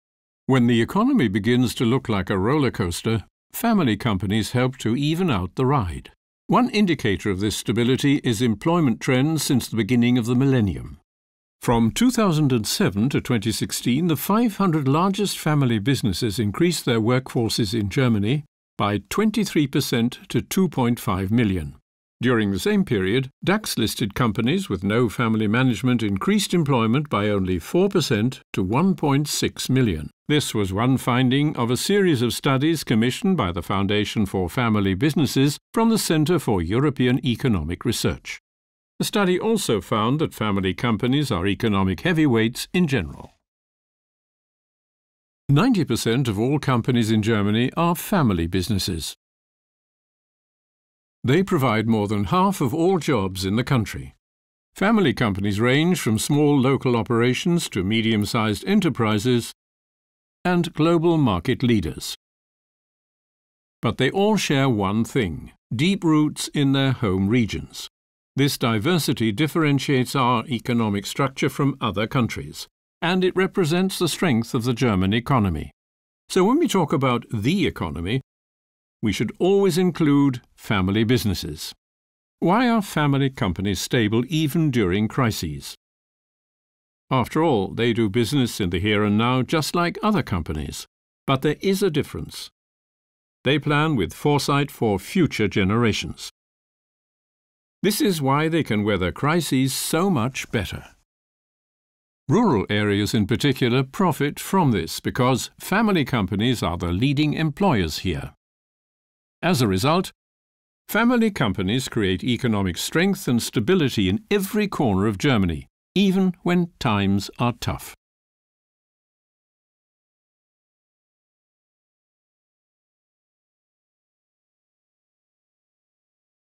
The economic significance of family businesses
For more than a decade now, the Foundation for Family Businesses has regularly commissioned the Centre for European Economic Research (ZEW) and the Institute for SME research and entrepreneurship in Mannheim (ifm) to conduct studies on the economic importance of German family businesses. Our short film summarises the key statistics, such as the share of the workforce and revenues accounted for by family businesses.